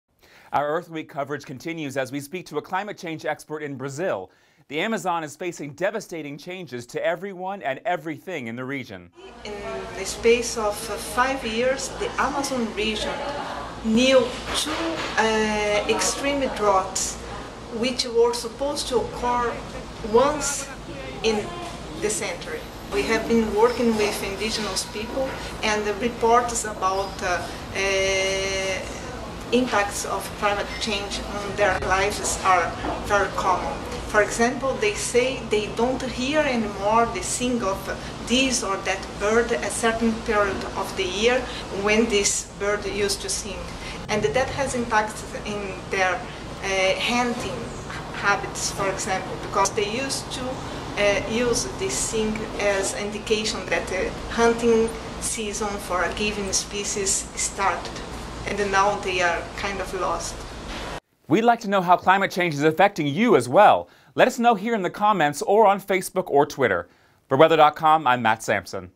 Earth Week: Amazon Dealing With Drought
As part of Earth week, we speak to an expert in Brazil about the challenges they face as part of global change.